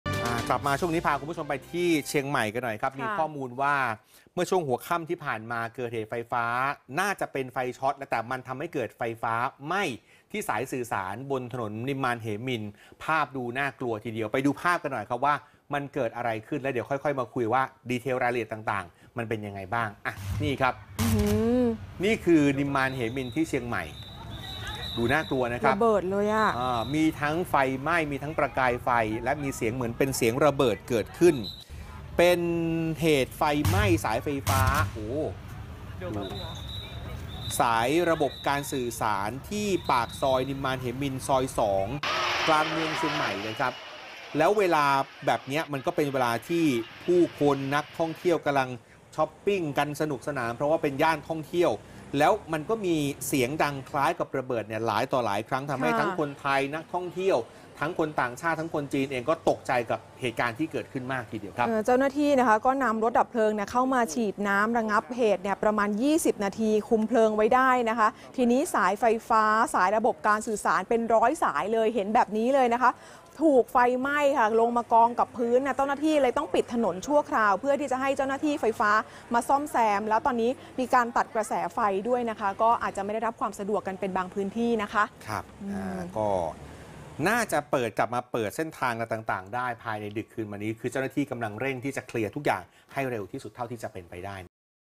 ไฟไหม้สายสื่อสารระเบิดเสียงดังสนั่นเมืองเชียงใหม่
เชียงใหม่ 18 มิ.ย.-ไฟไหม้สายสื่อสาร ย่านถนนนิมมานเหมินทร์กลางเมืองเชียงใหม่ ประชาชนและนักท่องเที่ยวต่างพากันแตกตื่นวิ่งหนีอลหม่าน

1.สารคดีเฉลิมพระเกียรติ
2.พระราชกรณียกิจ-พระมหากรุณาธิคุณ
3.คนไทยหัวใจ♥รักในหลวง